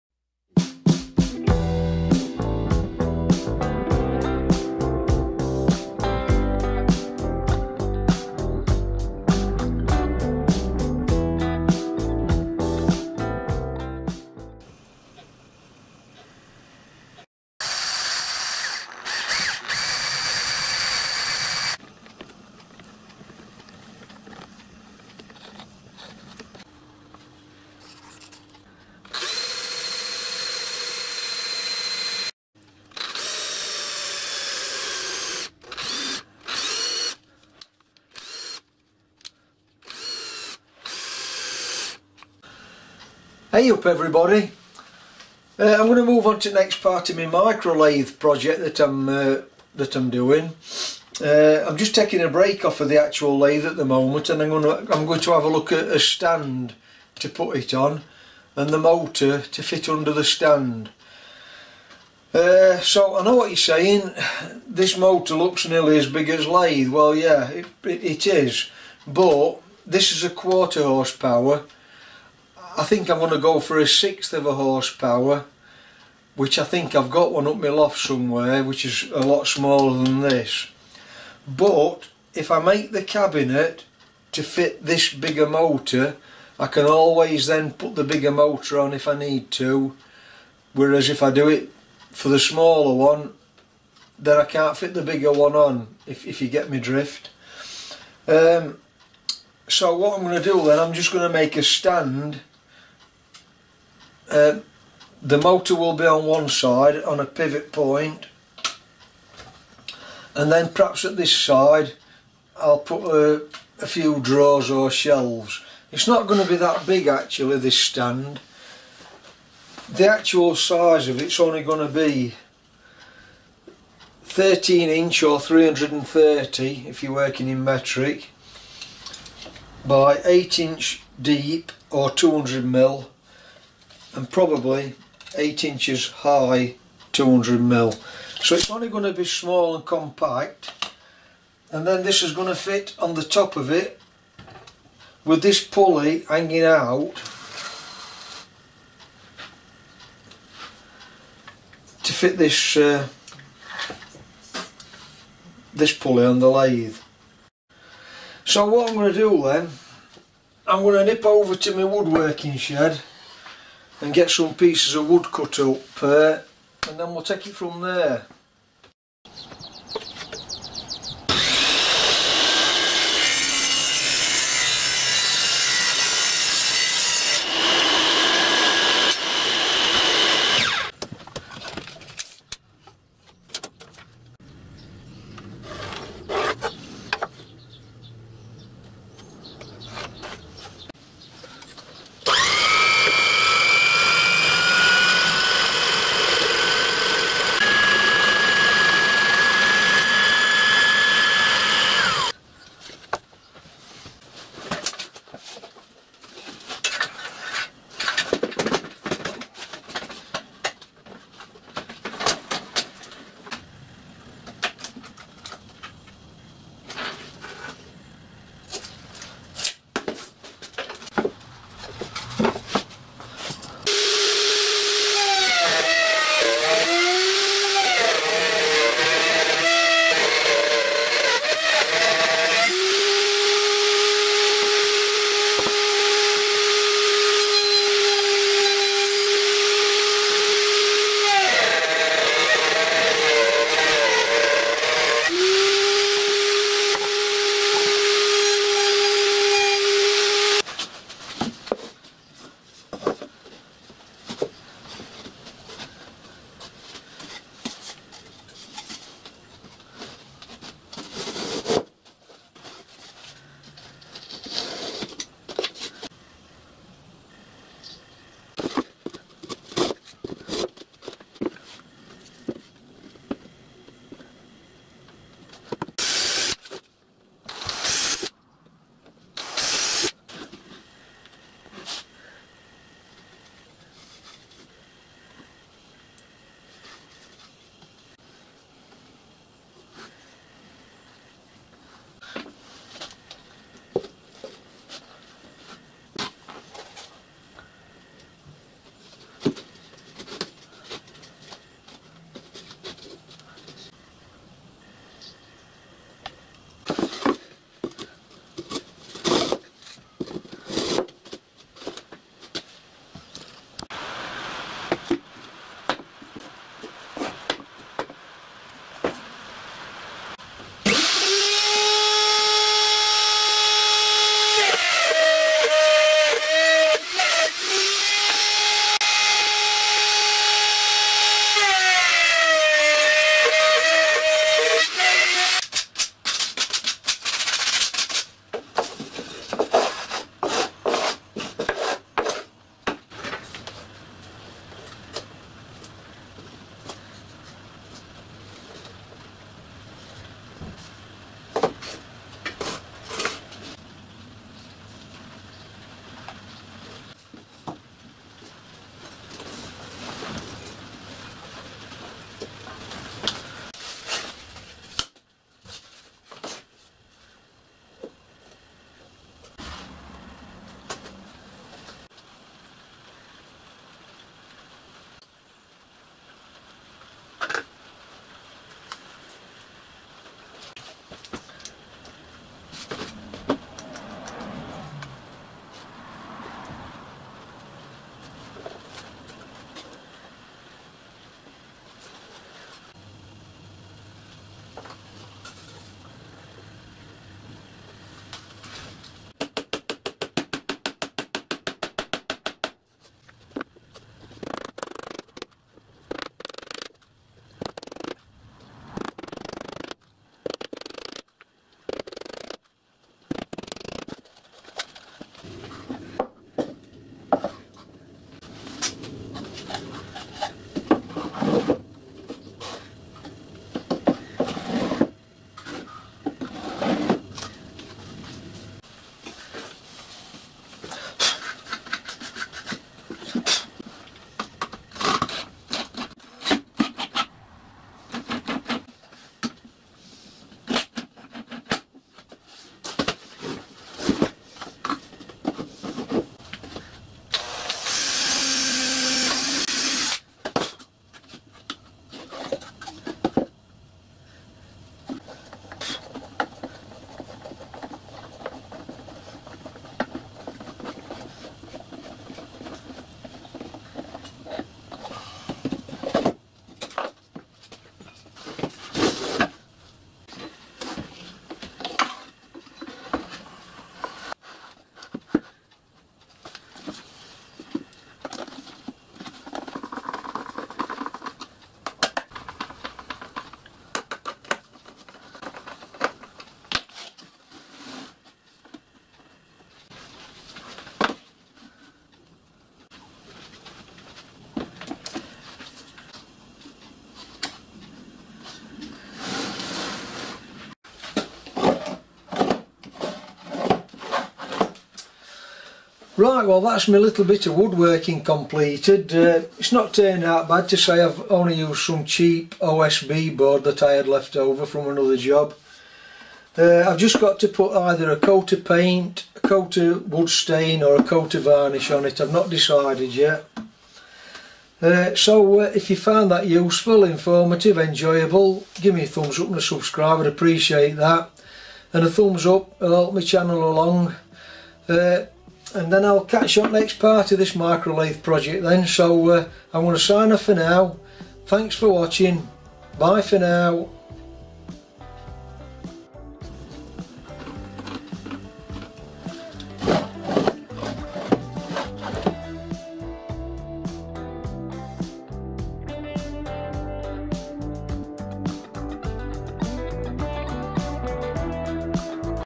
Ep 14 Making a Micro / Mini / Watchmakers Lathe from Old Casting (Temporary Cabinet ) "mr factotum"
Episode 14 Here Showing Making a Temporary Cabinet /Stand to house the Motor and Mount the Lathe on. Designing and Making from Scratch so i can get the motor powered up tp check everything and to mark the centre of the Tailsock casting.
 This lathe was briefly started many many years ago by an unknown Engineer, passed through a few pairs of hands since with nothing else done to it, until i got hold of it, so i will continue with the small amount of work already done, to finish the lathe in to a Imperial  Micro / Mini / Watchmakers / small Lathe with screw cutting capabilities & back geared, everything has got to be made from Scratch including the Back Gears, Clutch, Change wheels, Leadscrew, Tailsock, Spindle, Pulley's, Screwcutting Mechanism, Dials etc etc just to name a few. The casting is very elegant and well made item, with a good foundation to start from.
There were numerous manufactures of these small lathe in Great Britain many moon's ago, to many to name here, so do a search and take a look at the huge seclection of makers and differing shapes, sizes and capabilities.
Kev